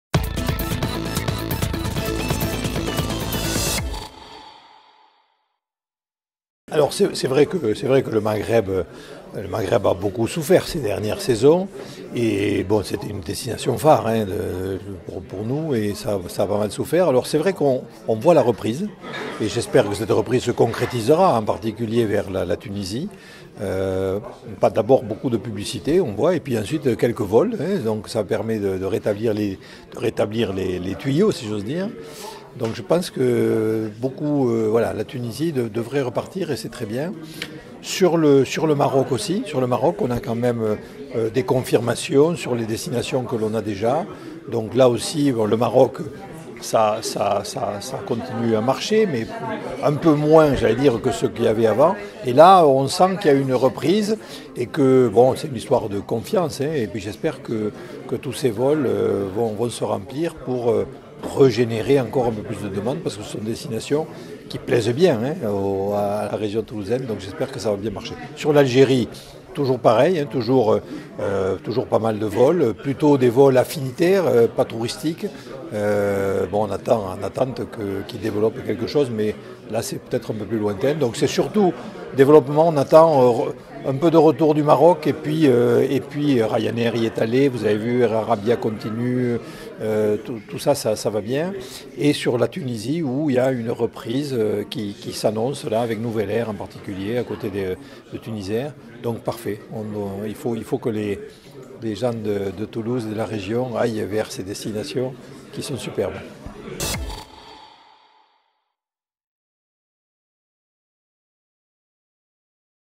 L'aéroport de Toulouse développe ses vols vers la Méditerranée
L'Aéroport de Toulouse Blagnac a présenté son catalogue de vols Printemps-Eté 2017. La plateforme toulousaine propose 10 destinations vers l'Afrique, avec une nouvelle ouverture de liaison avec Djerba. Le point en vidéo avec Jean-Michel Vernhes, président du directoire de l'Aéroport Toulouse-Blagnac.